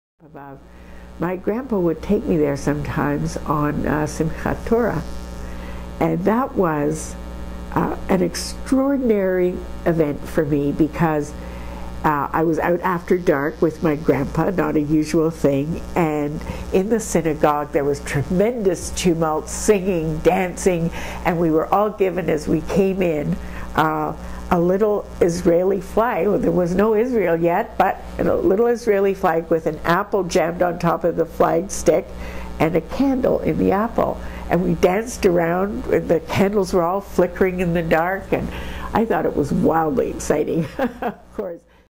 Interview with Michele Landsberg
Canadian author and journalist Michele Landsberg provides recollections of attending the Minsk Synagogue with her grandfather in the 1940s.

Oral history: Michele Landsberg, August 2006. Ontario Jewish Archives, AC 294.